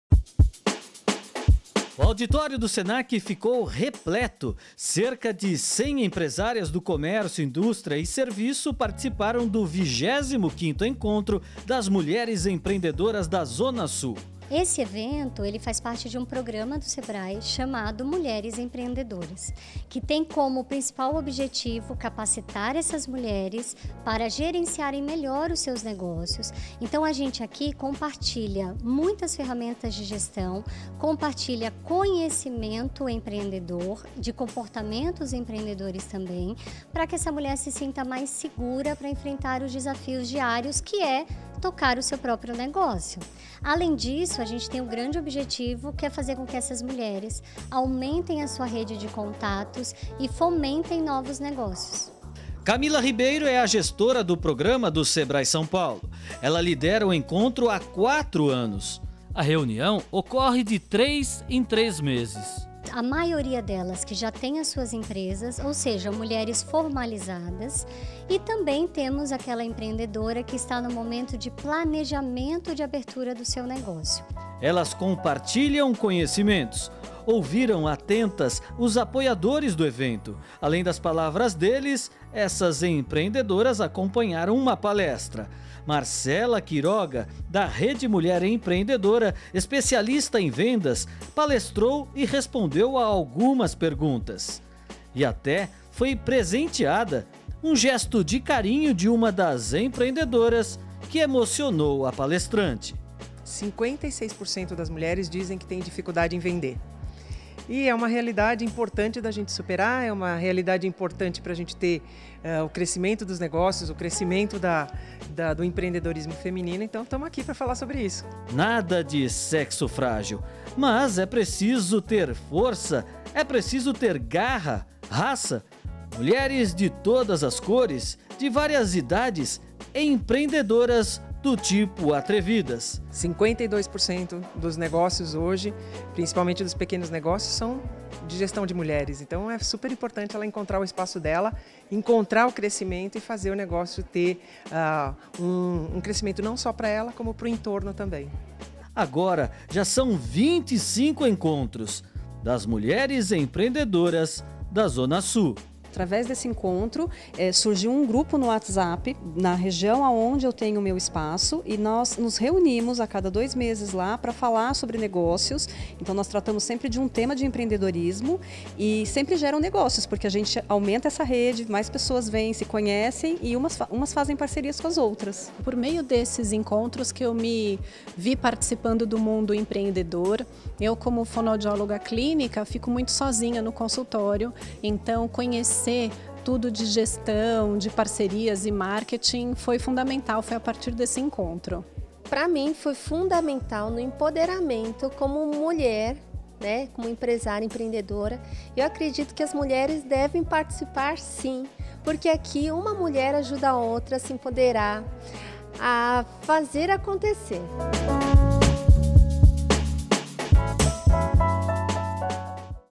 Sebrae-SP realiza 25º Encontro de Mulheres Empreendedoras da Zona Sul
O Programa Mulheres Empreendedoras tem por objetivo auxiliá-las no desenvolvimento de suas empresas, ampliando sua visão de negócios e potencial de mercado através do conhecimento, sugestão de aplicação de ferramentas e incentivo à autonomia em decisões.